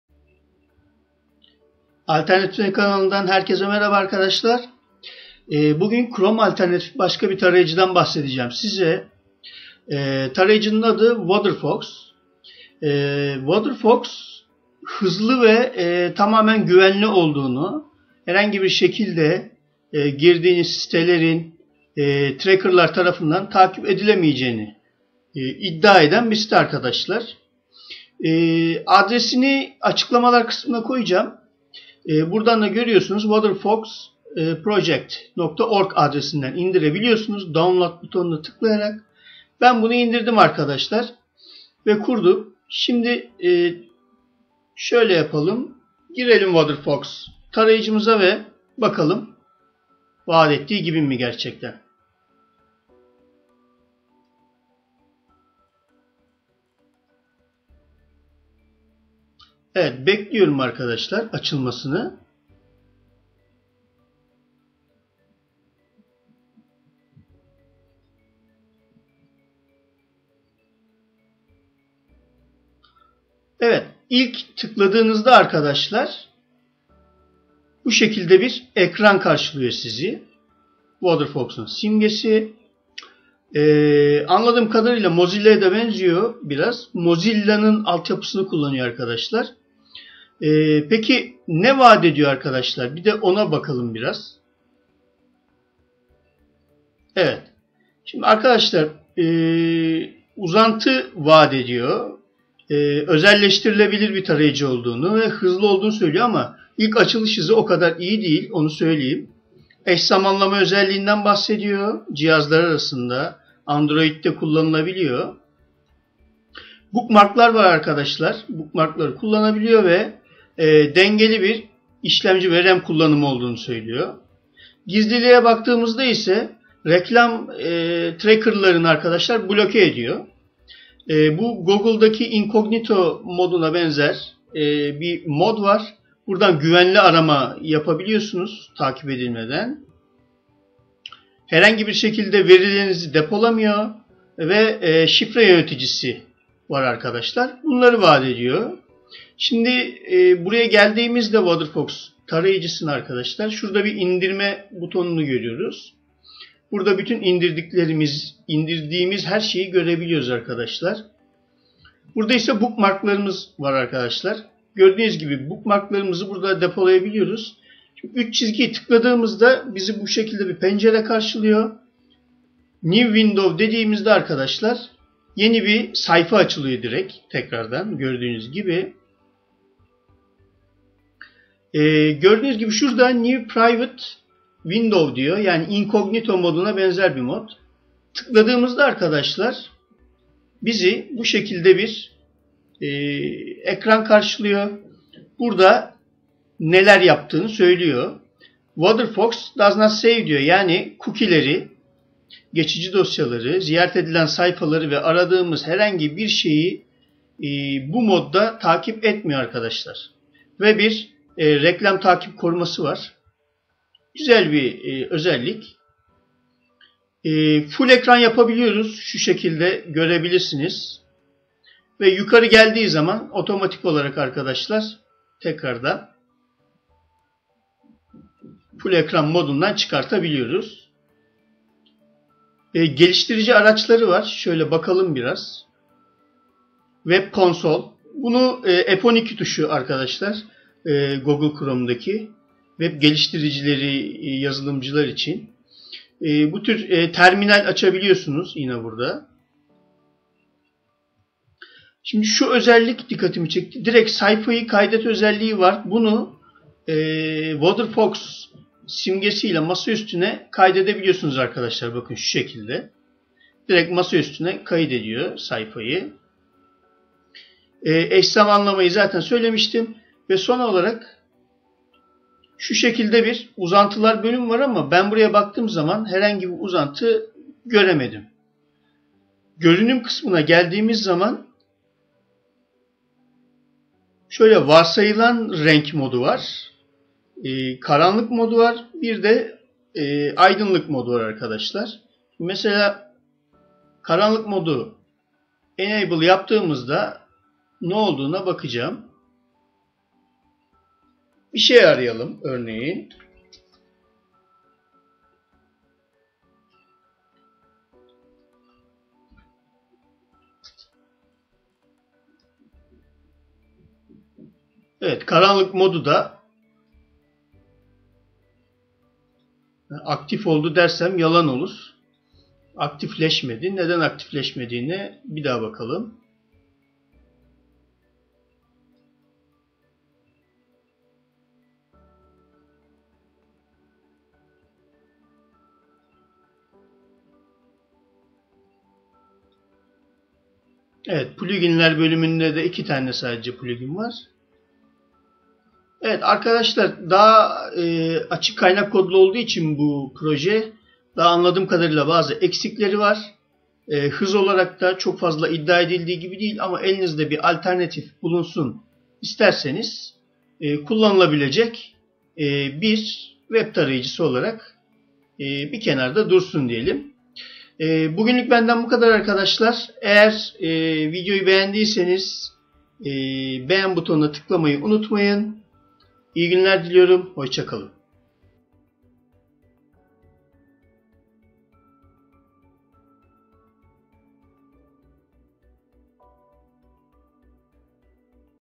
Chrome Alternatifleri: Waterfox Browser
Web tarayıcıları na değindiğim bu seride bir browser olarak Waterfox Web Browser üzerine bir video çekmek istedim. Videoda Waterfox un bazı özelliklerini incelerken Google Chrome ile de ufak karşılaştırmalar yaparak "Alternatifi Ne?" Sorusuna da yanıt vermeye çalıştım..

Herkese keyifli seyirler...